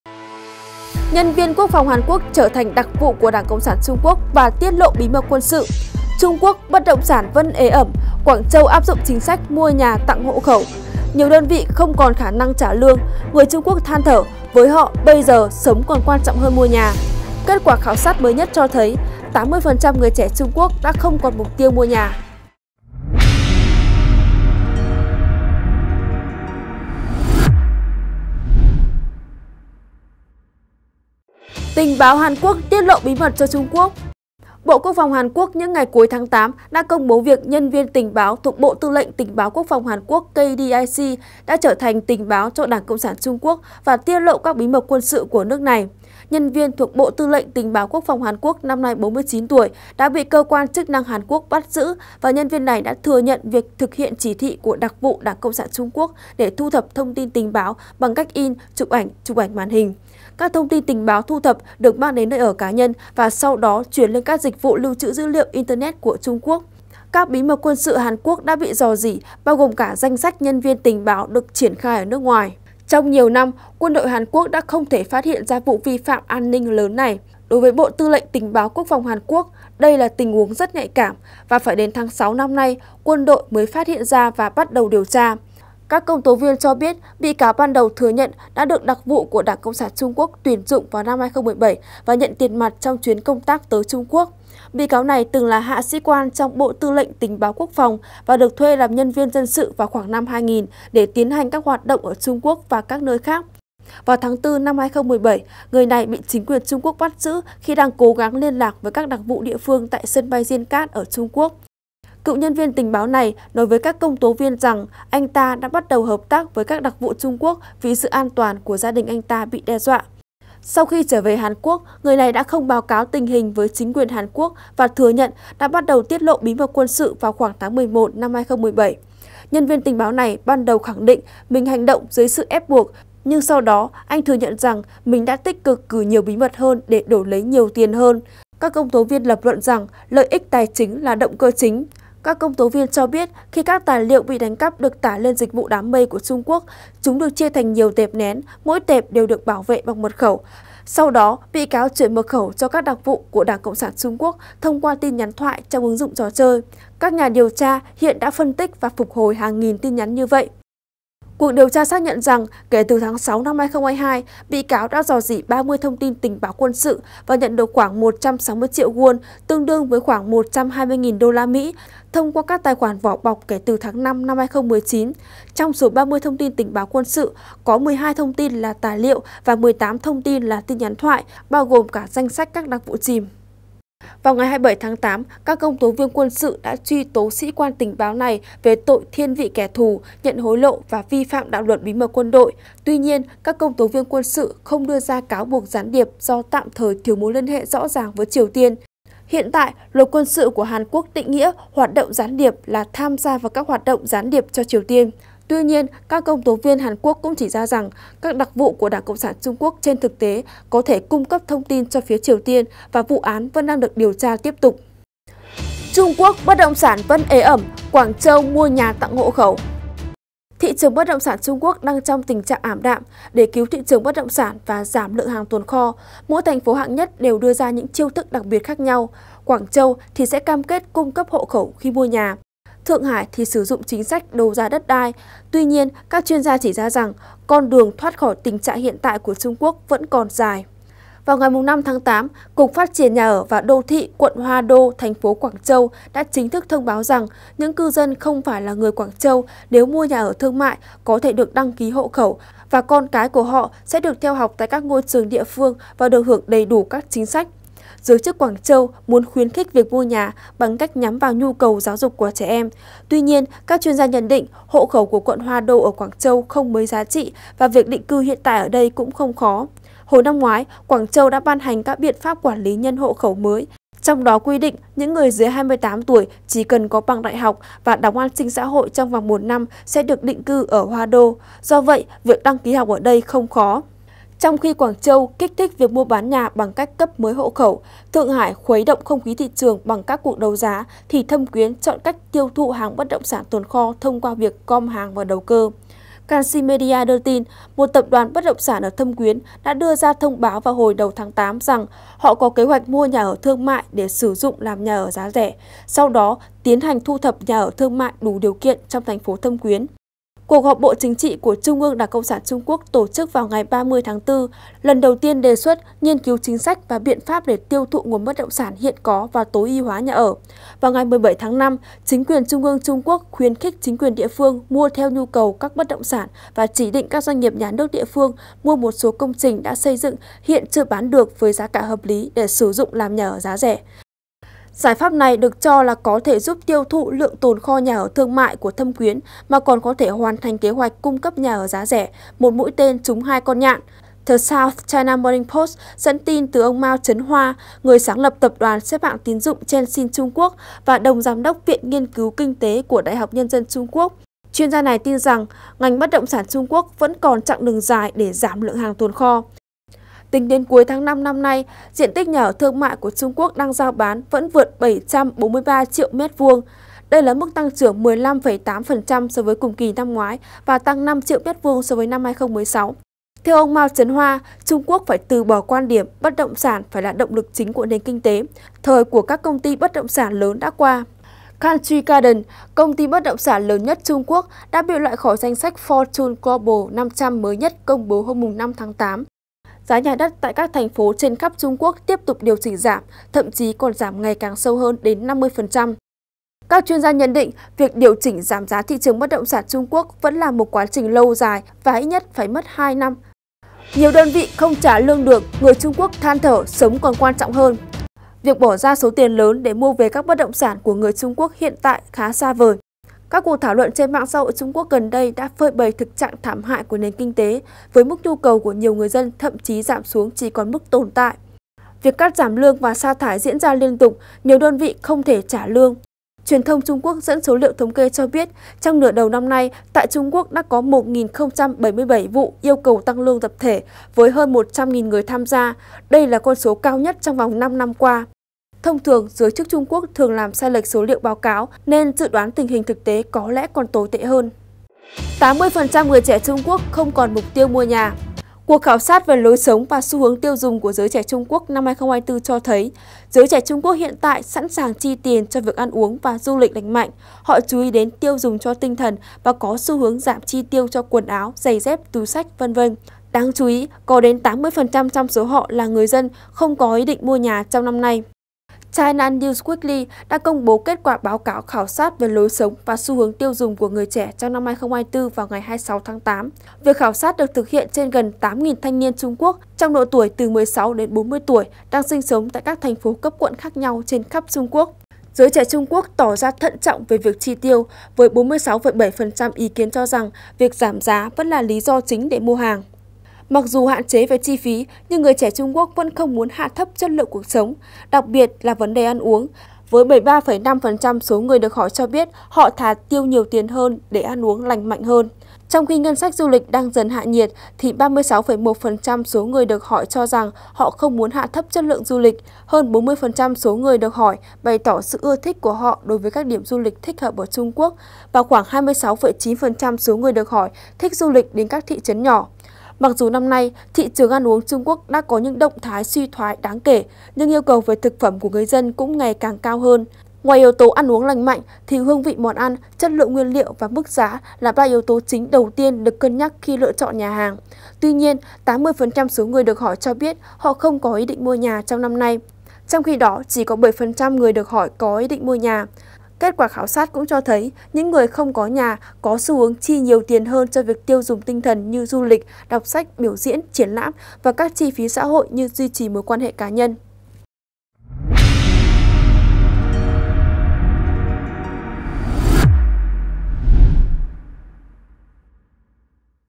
Nhân viên Quốc phòng Hàn Quốc trở thành đặc vụ của ĐCSTQ và tiết lộ bí mật Quân sự
- Trung Quốc bất động sản vẫn ế ẩm, Quảng Châu áp dụng chính sách “mua nhà tặng hộ khẩu”
- Nhiều đơn vị không còn khả năng trả lương, người Trung Quốc than thở với họ bây giờ sống còn quan trọng hơn mua nhà.
- Kết quả khảo sát mới nhất cho thấy, 80% người trẻ Trung Quốc đã không còn mục tiêu mua nhà
✿ Bật chuông 🔔 để nhận thông báo và là người đầu tiên xem bản tin của DBC News

Sự tin tưởng và yêu thích của quý vị là động lực để chúng tôi cố gắng và phục vụ tốt hơn. Chân thành cảm ơn quý vị!